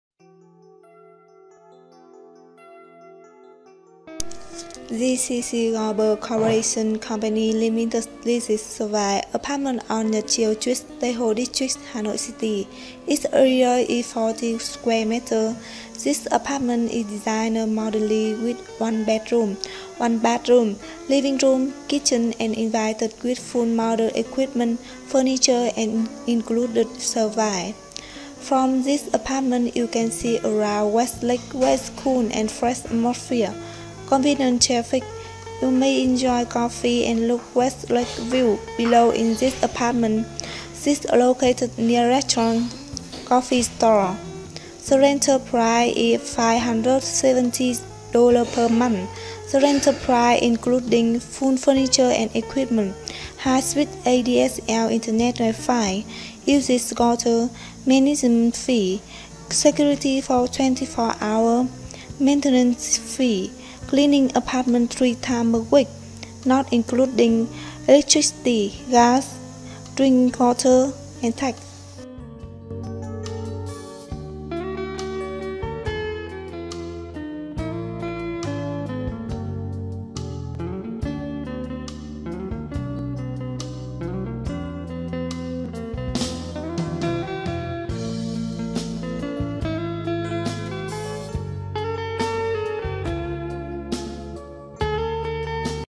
Studio Apartment In Tay Ho District, Hanoi City
GCC Global Cooperation Company Limited leases serviced apartment on Nhat Chieu Street, Tay Ho District, Hanoi City. Its area is 40sqm. This apartment is designed modernly with 1 bedrooms, 1 bathroom, living room, kitchen and invested with full modern equipment, furniture and included services. From this apartment you can see around West Lake where is cool and fresh atmosphere, convenient traffic. You may enjoy coffee and look West Lake’s view below in this apartment. It is located near restaurants, coffee stores.

The rental price is 570 $ per month

The rental price including :
- Full furniture and equipment
- High speed ADSL Internet, wifi
- Usage water
- Management fee
- Security for 24 hours
- Maintenance fee
- Cleaning apartment 3 times per week .

Not including :
- Electricity
- Gas
- Drinking water
- Tax

GCC GLOBAL CO.,LTD
Office Adress: 5th floors, No.128, Trung Kinh street, Cau Giay district, Hanoi City, Vietnam.
Skype: batdongsan.gcc